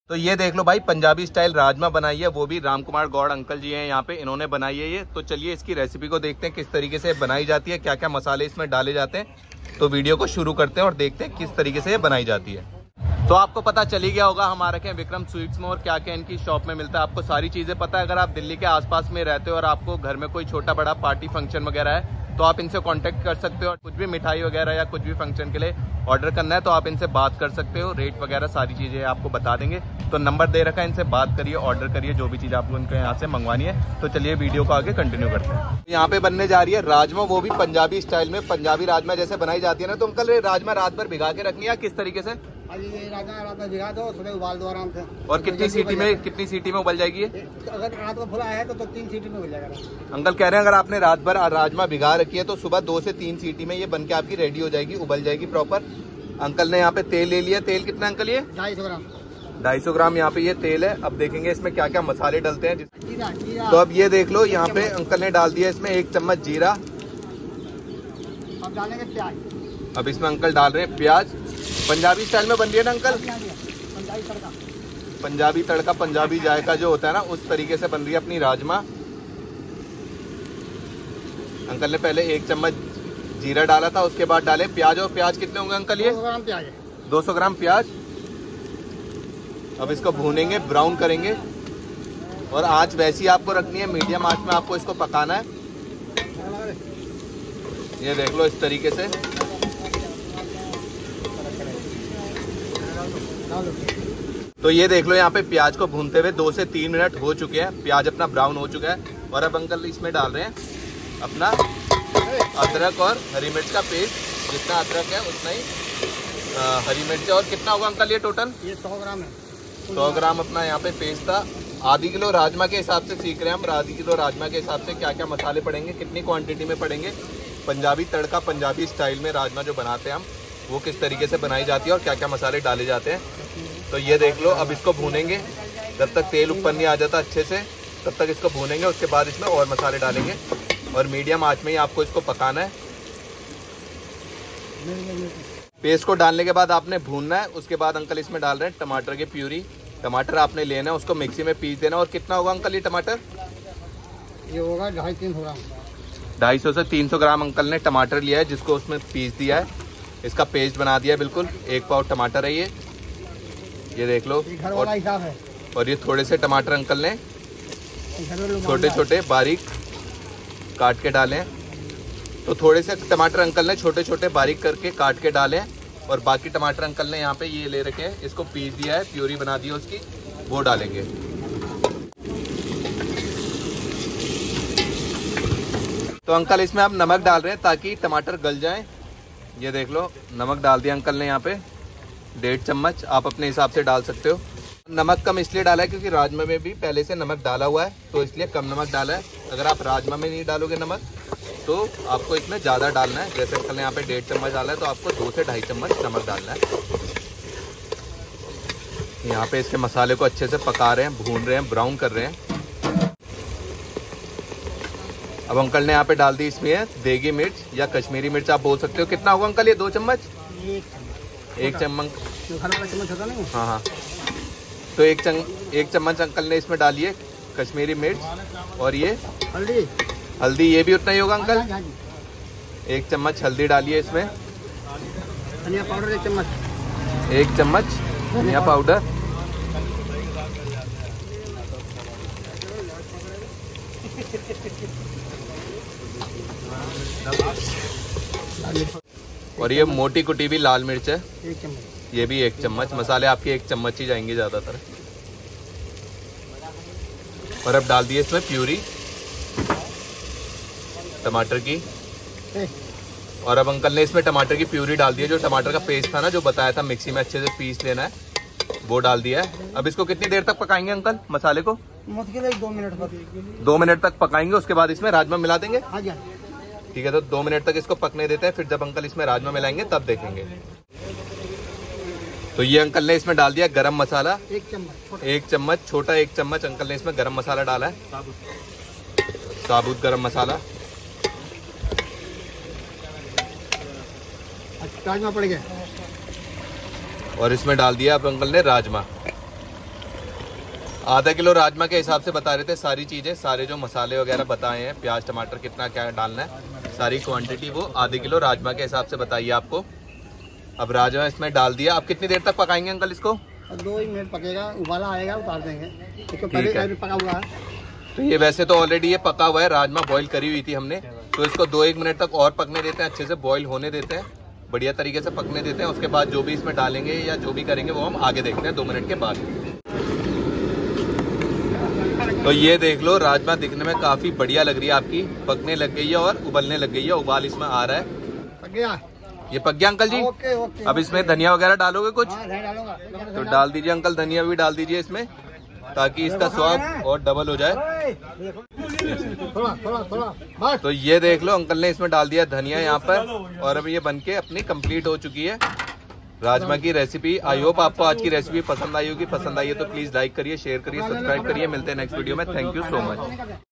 ऐसे बनाओ घर पर शादी वाला पंजाबी राजमा मसाला , घर वाले भी बोलेंगे कहा से सिखा -RAJMA MASALA Recipe
Vikram Sweets & Catering
Address - Kautilya Marg , Chanakyapuri Opposite Gujrat Bhawan Delhi

Rajma
Rajma recipe
Rajma kaise banaye
Punjabi rajma masala